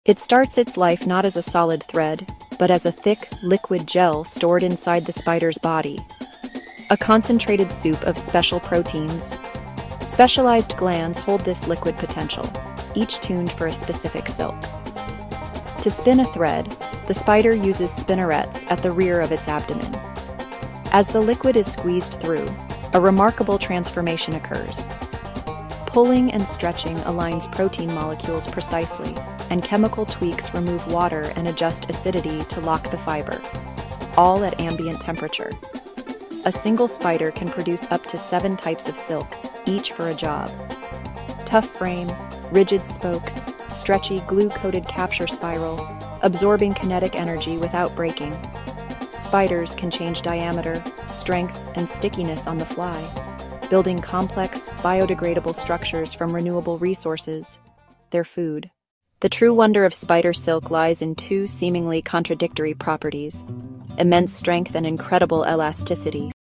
I Tried Making Spider Silk From Scratch
Uncover the secrets of spider silk! This video dives into its unique creation, remarkable properties, and versatile applications. It's stronger than steel and lighter than cotton!